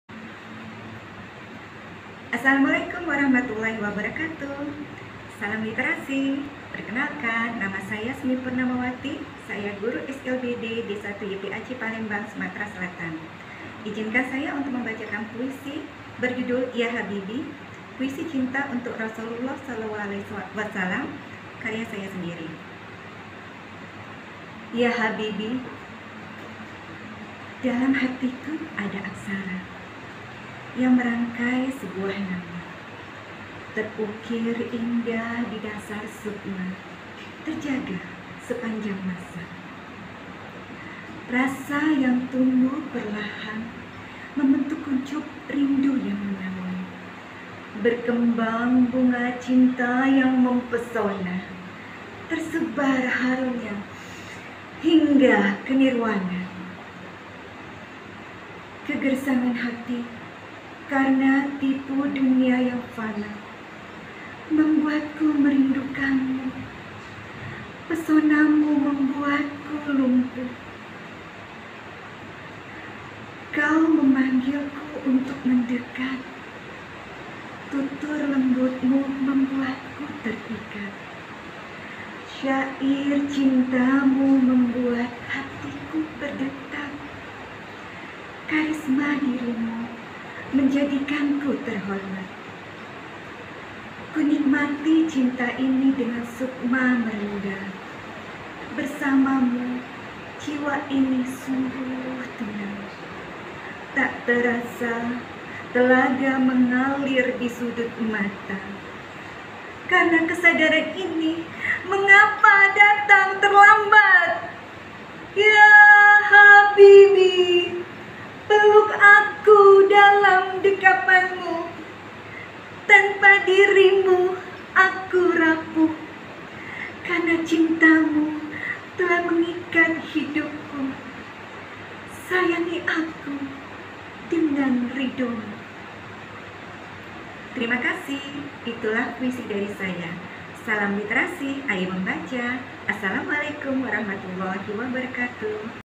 Judul Puisi: "YA HABIBI", oleh: Semi Purnamawati, S.Pd.
FestivalNusantaraBerpuisi SLB_YPAC_Palembang, provinsi Sumatera Selatan.